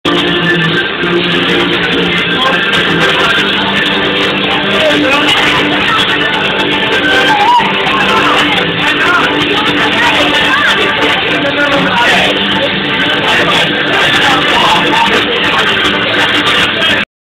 Happy New Year
Not staged, some dude at a party NYC, 2008.